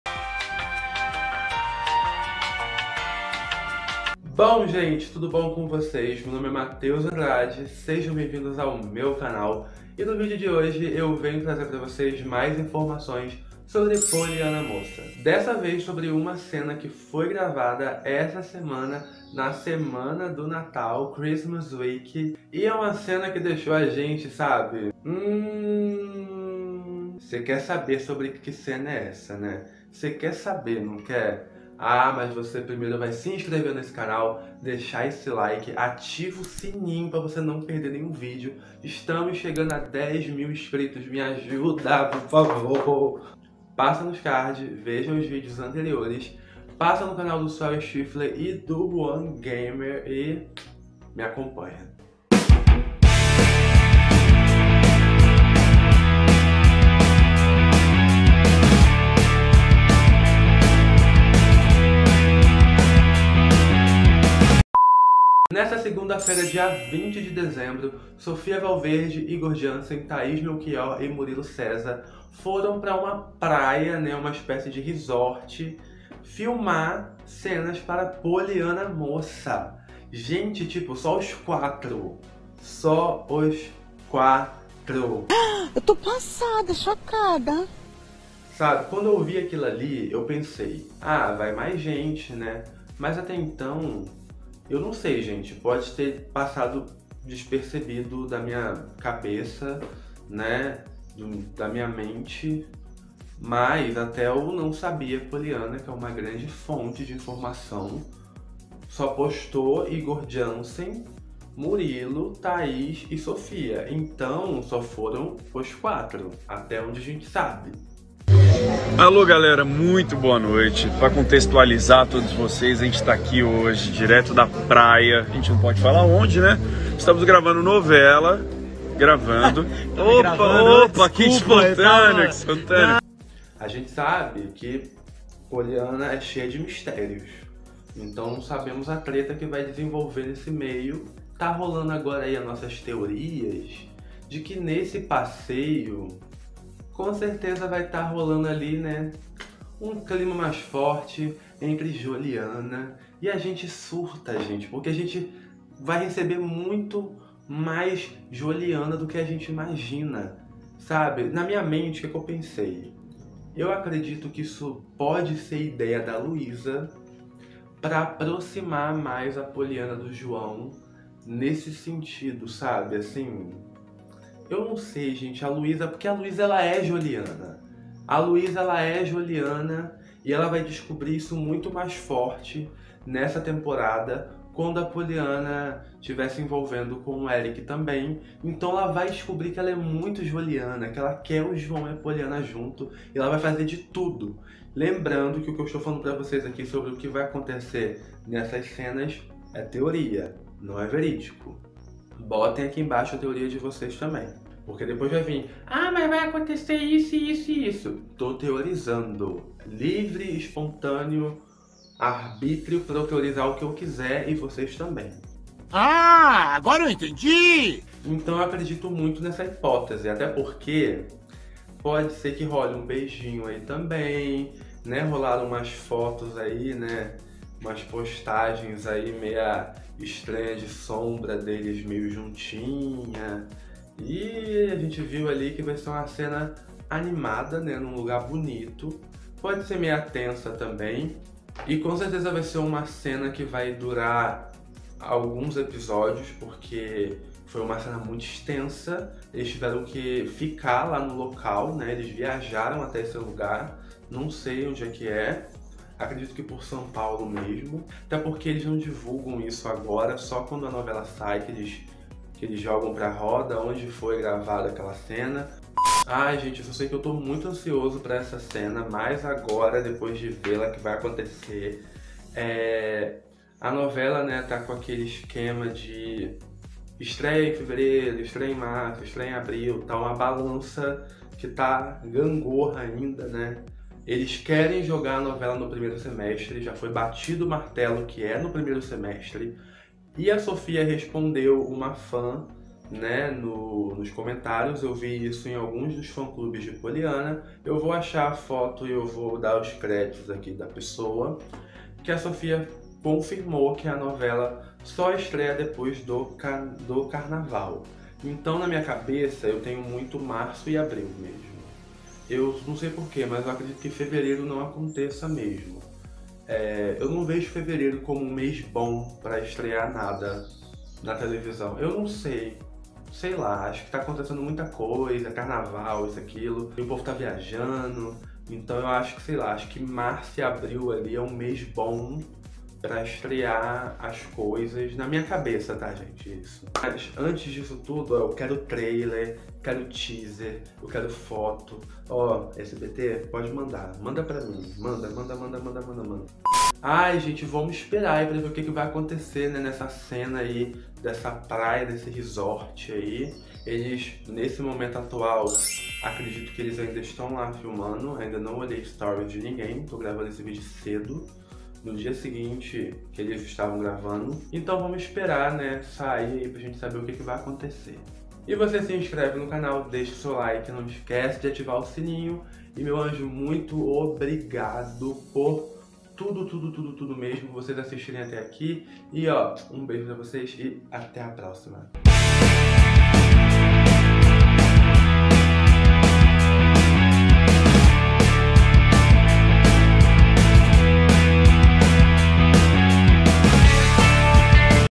POLIANA MOÇA: LUCELO E JOLIANA NA PRAIA EM ENCONTRO ROMÂNTICO? O QUE ROLOU? | Mateus Andrade
Na semana do natal eu gravei um vídeo sobre as cenas que Thaís Melchior, Murilo Cezar, Igor Jansen e Sophia Valverde gravaram em uma praia cenas para Poliana Moça.